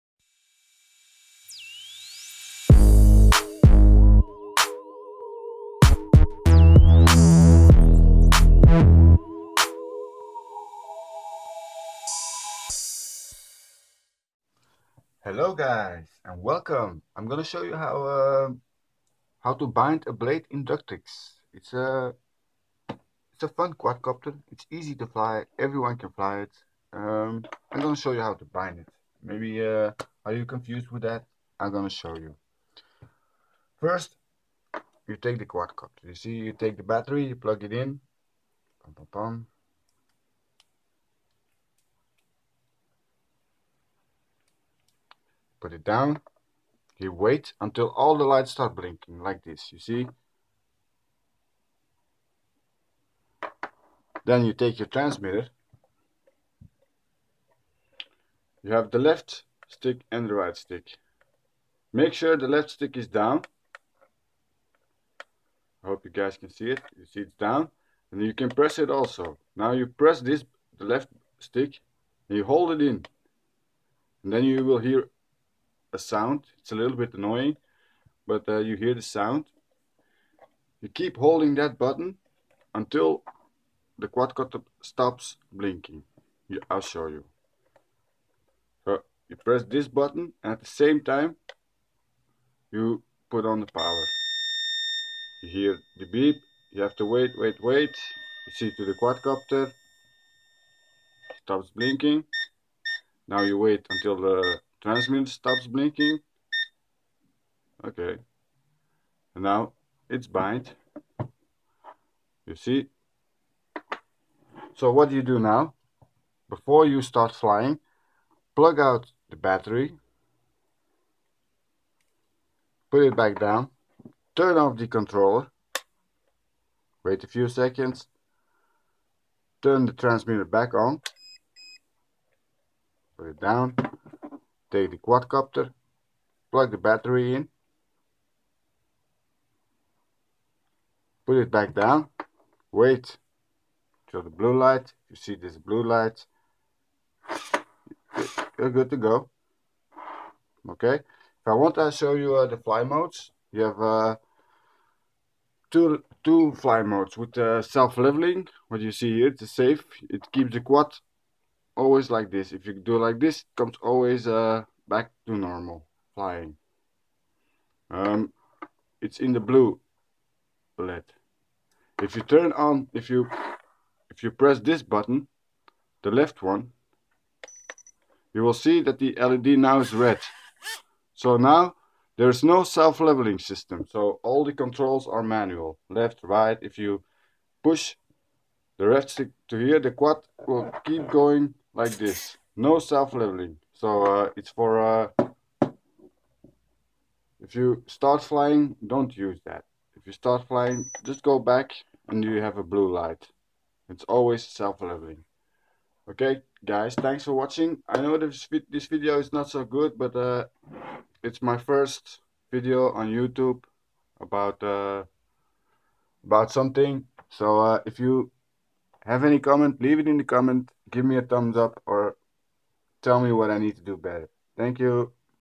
Blade inductrix setup video
My first how to video. I know it sucked but you need to start somewhere. ;-) hint tips are always welcome. Have a nice vieuw :p